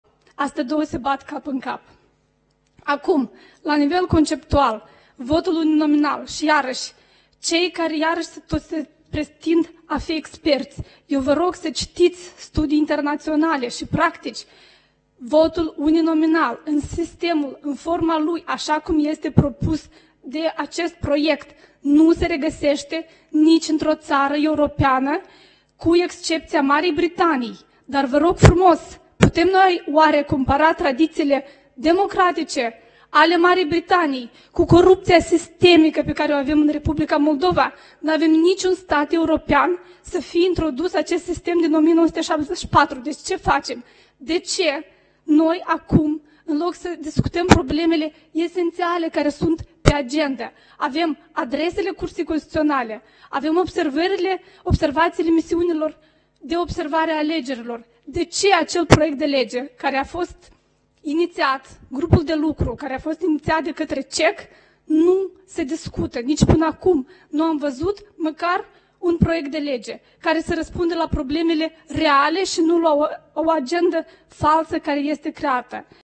Audieri publice privind reforma sistemului electoral, inițiativa parlamentară pr...
Putem compara tradițiile democratice  ale Marii Britaniei cu corupția sistemică pe care o avem &#238;n Republica Moldova?

Titlul evenimentului: Audieri publice privind reforma sistemului electoral, inițiativa parlamentară privind sistemul uninominal, revizuirea și modificarea Codului electoral și a legislației conexe